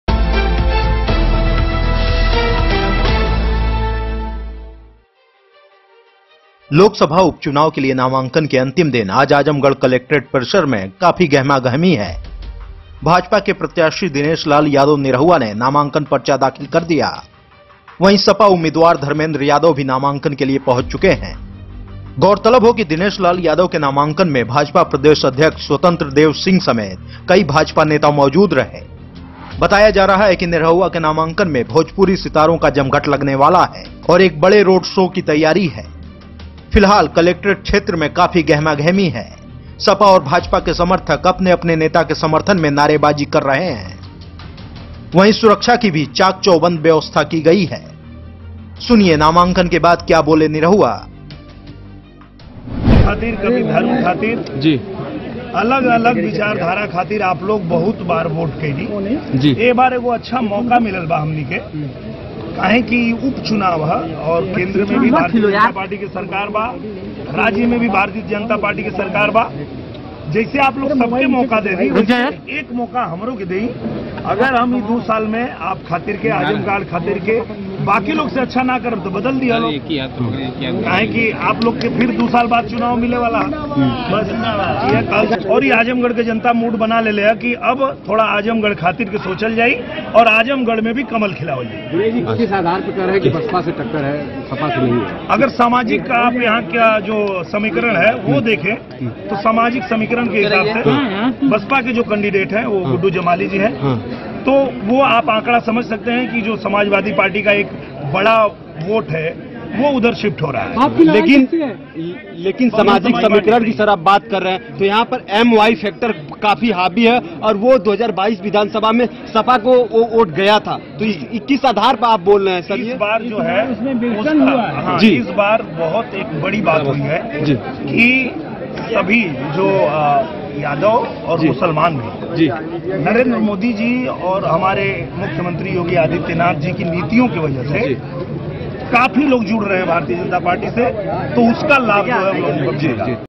BJP के निरहुआ ने किया आजमगढ़ से नामांकन, सपा को बता दिया तीसरे नंबर की पार्टी!
लोकसभा उपचुनाव के लिए नामांकन के अंतिम दिन आज आजमगढ़ कलेक्ट्रेट परिसर में काफी गहमागहमी है। भाजपा के प्रत्याशी दिनेश लाल यादव निरहुआ ने नामांकन पर्चा दाखिल कर दिया। वहीं सपा उम्मीदवार धर्मेंद्र यादव भी नामांकन के लिए पहुंच चुके हैं। वहीं दिनेश लाल यादव के नामांकन में भाजपा प्रदेश अध्यक्ष स्वतंत्र देव सिंह समेत कई भाजपा नेता मौजूद रहे।